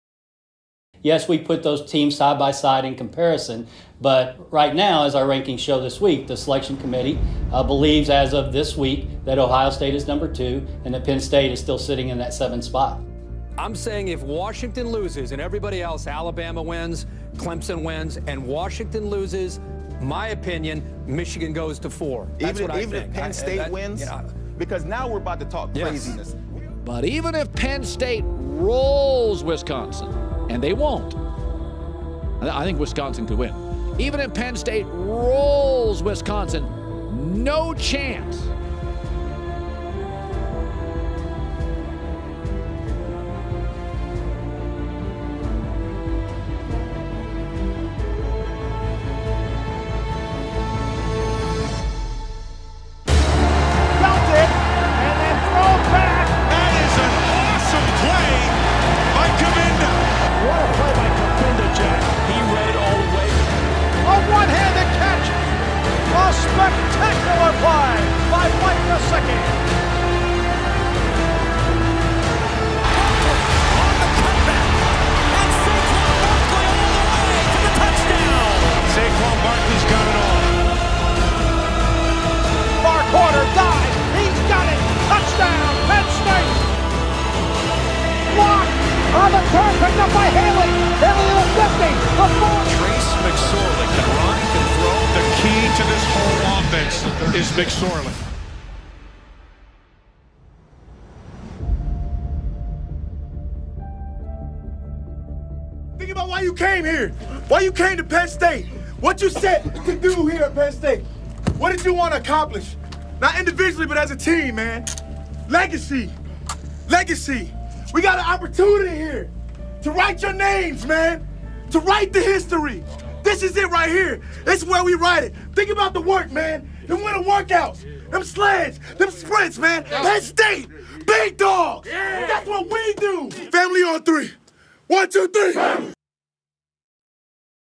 No more doubts - Penn State B1G Championship HYPE
It's time to prove the doubters wrong again. Penn State vs Wisconsin in the B1G Championship Game 2016.